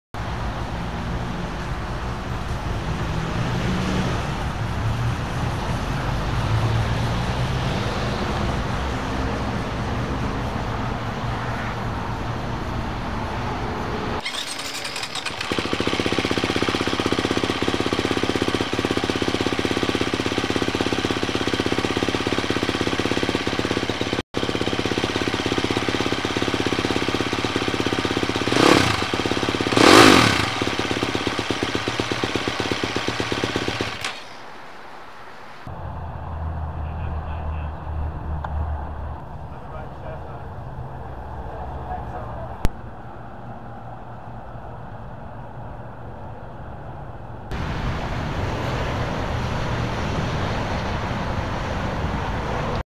2021 Husqvarna FX 350 | Walkaround
Look at our Husqvarna FX 350 dirt bike. In this walk around you can see the sleek colors: white, green and blue. This bike looks great and feels great.